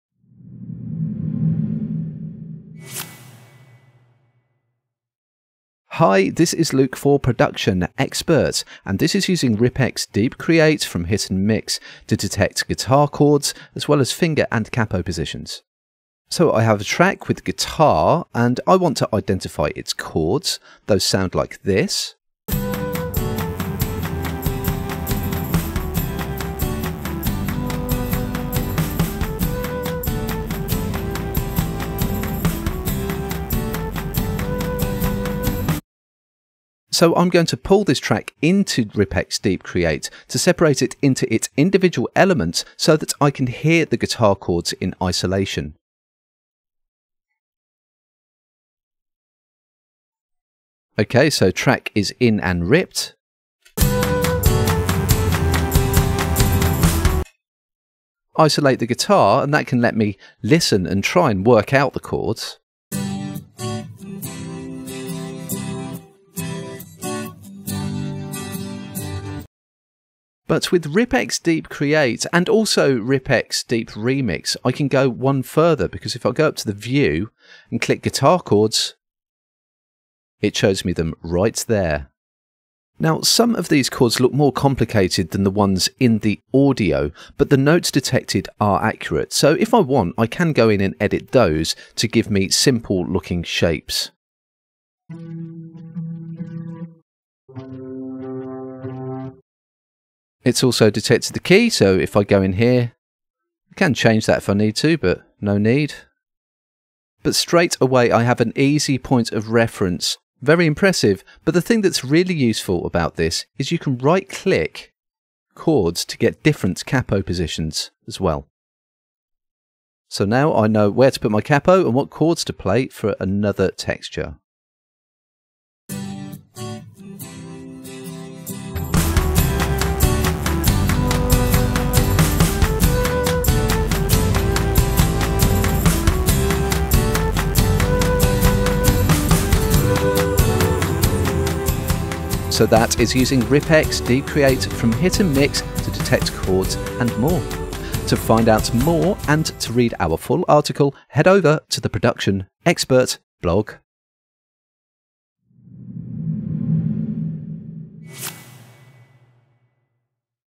Detecting Guitar Chords With RipX DeepCreate
With Hit’n’Mix’s audio tools offering repair and stem separation among other things, a recent addition to their RipX platform brings some musical tricks as well. We check out its musical ear for guitarists when the tab or music is missing.

Head over to the Production Expert blog to find out more about RipX's chord detection.…